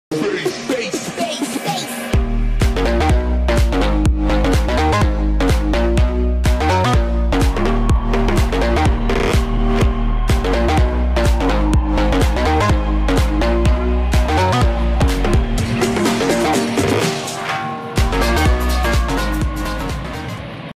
James Parsons scores against Altrincham Aces
The Deeside Dragons hit the silverblades arena and defeated Altrincham Aces 8-1. This was one of the goals scored by Parsons